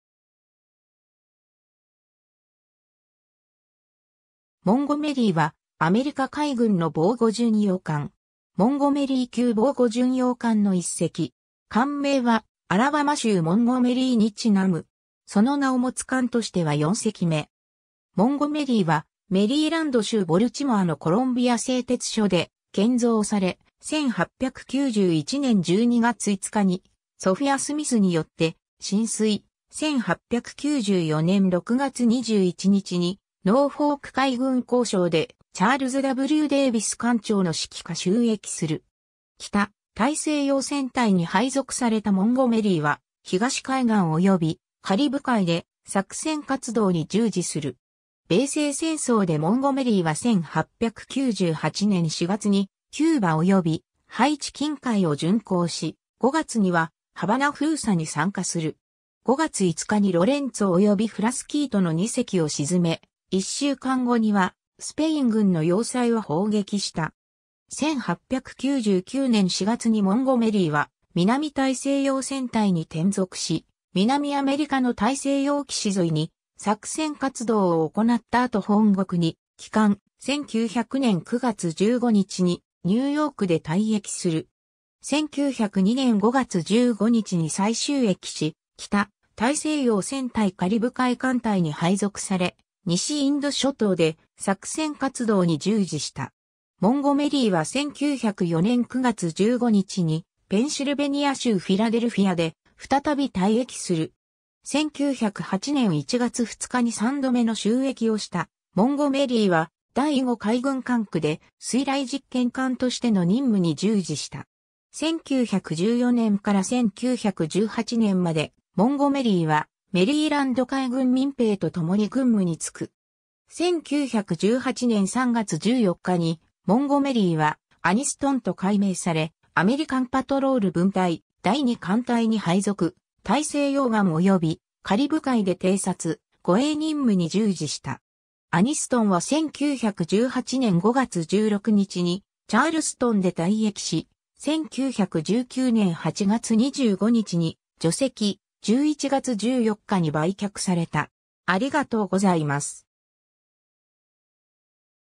モンゴメリー (防護巡洋艦)
モンゴメリー (USS Montgomery, C-9) は、アメリカ海軍の防護巡洋艦。
モンゴメリー級防護巡洋艦の1隻。
艦名はアラバマ州モンゴメリーに因む。
 モンゴメリーはメリーランド州ボルチモアのコロンビア製鉄所で建造され、1891年12月5日にソフィア・スミスによって進水、1894年6月21日にノーフォーク海軍工廠でチャールズ・W・デイヴィス艦長の指揮下就役する。
 北大西洋戦隊に配属されたモンゴメリーは東海岸及びカリブ海で作戦活動に従事する。
米西戦争でモンゴメリーは1898年4月にキューバ及びハイチ近海を巡航し、5月にはハバナ封鎖に参加する。
5月5日にロレンツォ (Lorenzo) およびフラスキート (Frasquito) の二隻を沈め、一週間後にはスペイン軍の要塞を砲撃した。
 1899年4月にモンゴメリーは南大西洋戦隊に転属し、南アメリカの大西洋岸沿いに作戦活動を行った後本国に帰還、1900年9月15日にニューヨークで退役する。
1902年5月15日に再就役し、北大西洋戦隊カリブ海艦隊に配属され、西インド諸島で作戦活動に従事した。
モンゴメリーは1904年9月15日にペンシルベニア州フィラデルフィアで再び退役する。
 1908年1月2日に三度目の就役をしたモンゴメリーは第5海軍管区で水雷実験艦としての任務に従事した。
1914年から1918年までモンゴメリーはメリーランド海軍民兵と共に軍務に就く。
1918年3月14日にモンゴメリーはアニストン (Anniston) と改名され、アメリカン・パトロール分隊第2艦隊に配属、大西洋岸及びカリブ海で偵察、護衛任務に従事した。
 アニストンは1918年5月16日にチャールストンで退役し、1919年8月25日に除籍、11月14日に売却された。